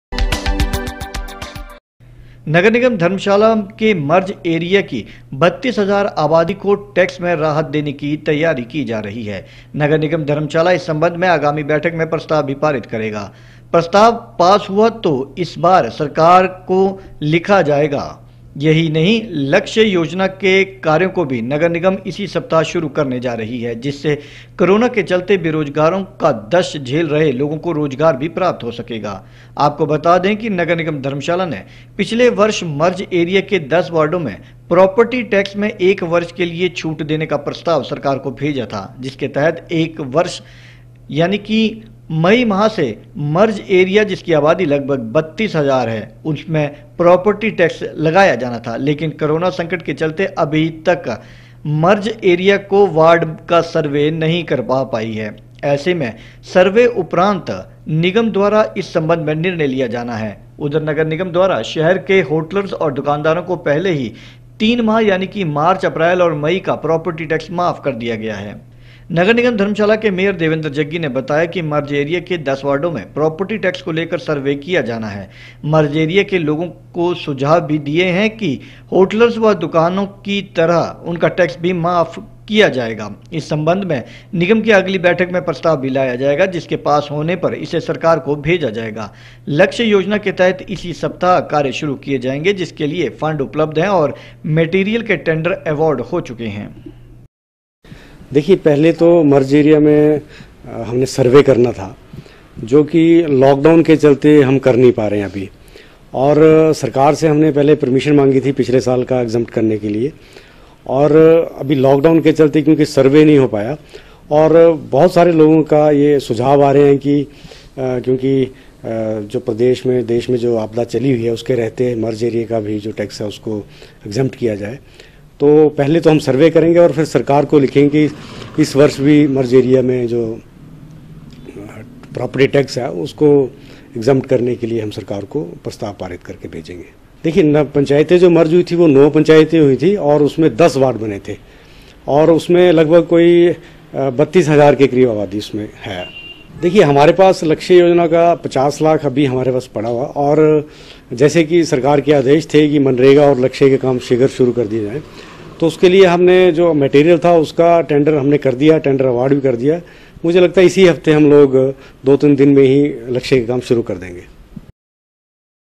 30 may 3 नगर निगम धर्मशाला के मर्ज एरिया की 32 हजार आबादी को टैक्स में राहत की तैयारी शुरू हो गई है
नगर निगम धर्मशाला के मर्ज एरिया की 32 हजार आबादी को टैक्स में राहत की तैयारी शुरू हो गई है। नगर निगम धर्मशाला इस संबंध में आगामी बैठक में प्रस्ताव लाएगी। प्रस्ताव पास हुआ तो इस बारे सरकार को लिखा जाएगा। यही नहीं लक्ष्य योजना के कार्यों को भी नगर निगम इसी सप्ताह शुरू करने जा रही है, जिससे कोरोना के चलते बेरोजगारी का दंश झेल रहे लोगों को रोजगार मिल सके।